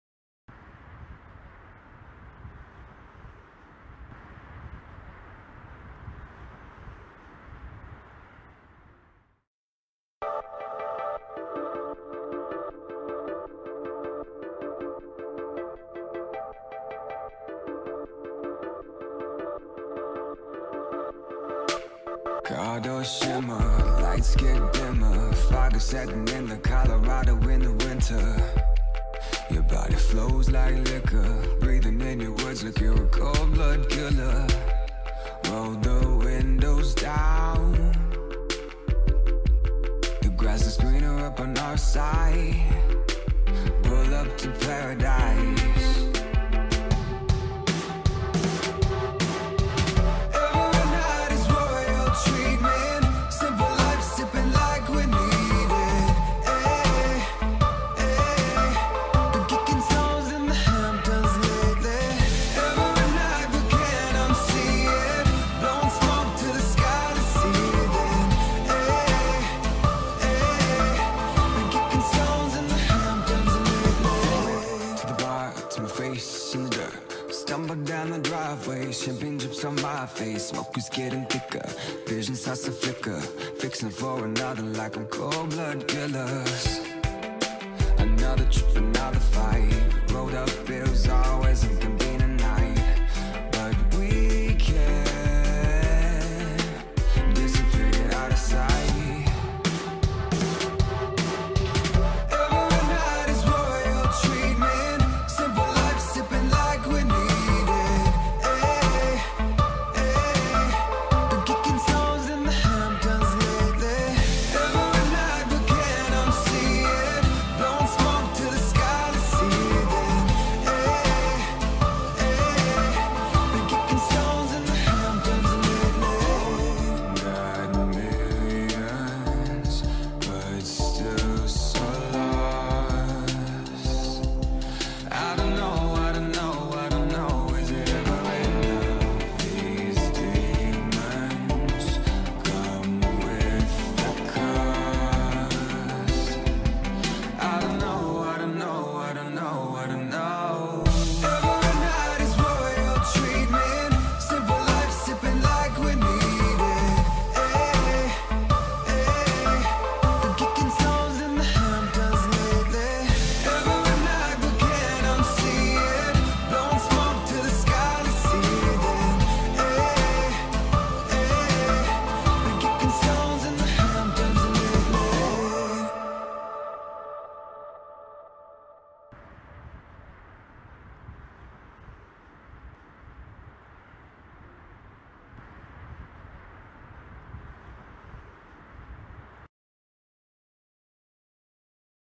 Spring in the Okanagan
No snow this spring in the Okanagan Valley and the mountain goats 🐑🐏 are adorable.